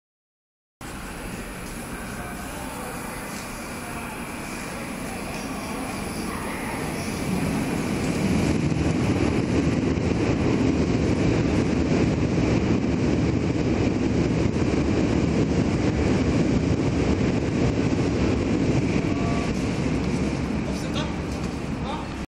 옥수역 ITX 청춘 통과영상
The ITX Chungchun train, passing through Oksu station